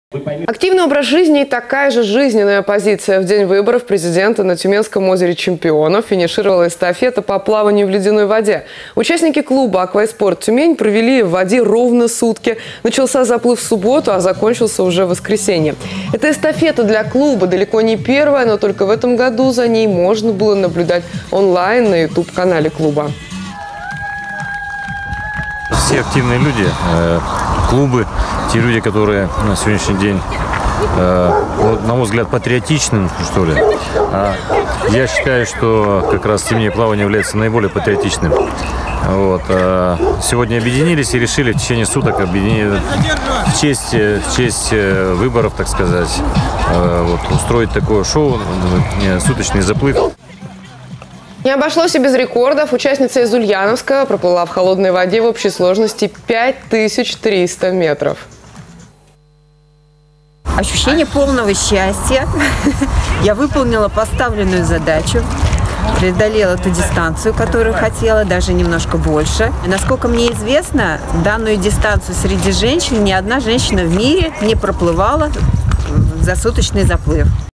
Заплыв длиной в сутки
В день выборов президента финишировала эстафета по плаванию в ледяной воде. Участники клуба «Аквайспорт Тюмень» провели в воде ровно сутки.
Алексей Салмин, вице-президент российской Федерации зимнего плавания: «Все активные люди, патриоты объединились в честь выборов и устроили суточный заплыв. Участница из Ульяновска проплыла в холодной воде в общей сложности 5 тыс. 300 метров.
Ольга Соколова, участница эстафеты, г. Уфа: «Выполнила задачу, преодолела дистанцию, которую хотела. Такую дистанцию еще никто из женщин в мире не проплывал за суточный заплыв».